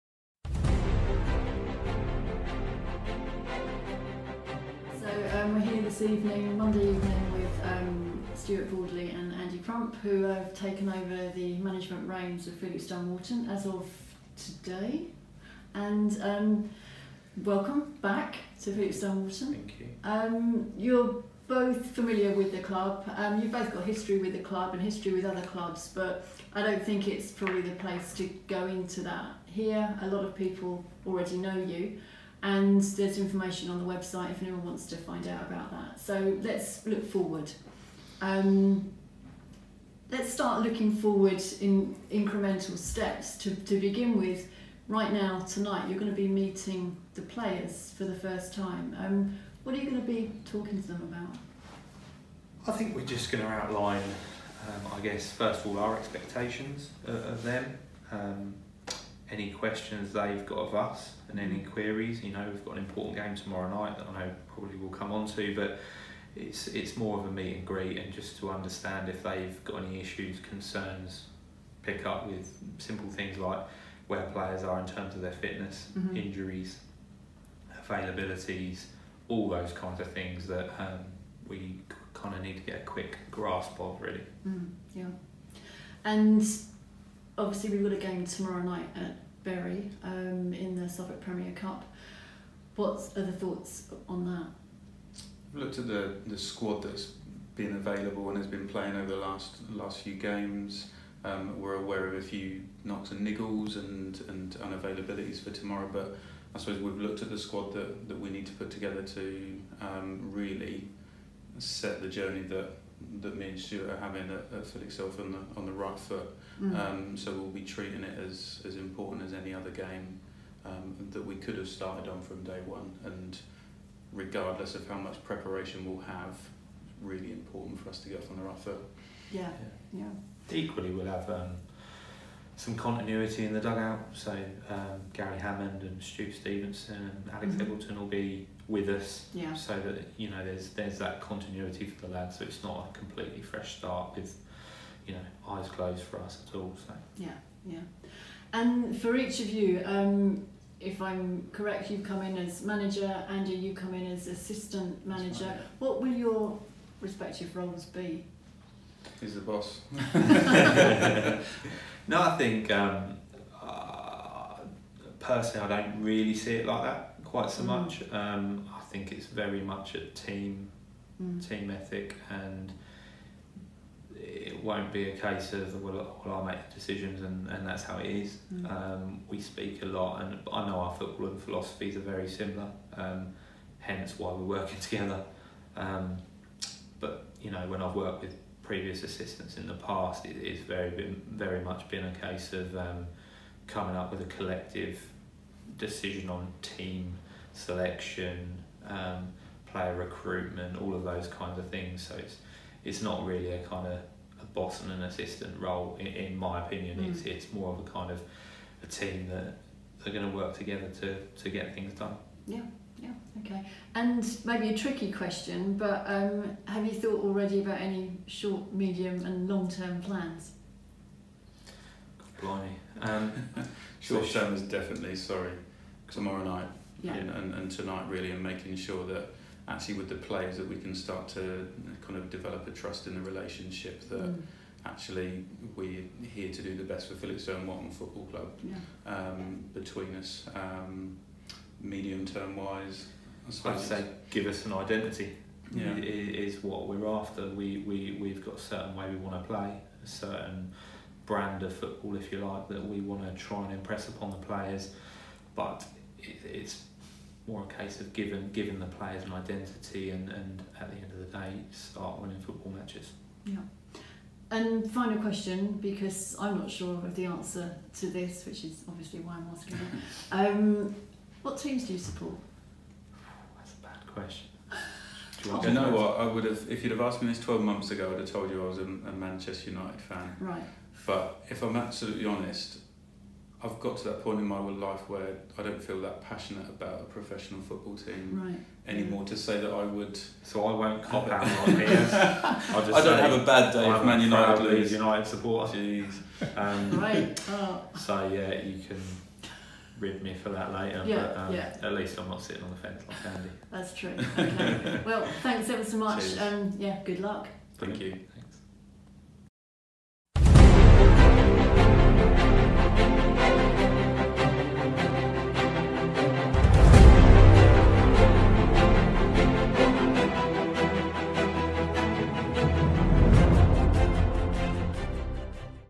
Interview with Stuart Boardley and Andy Crump
New management team Stuart Boardley and Andy Crump talk to Seasiders TV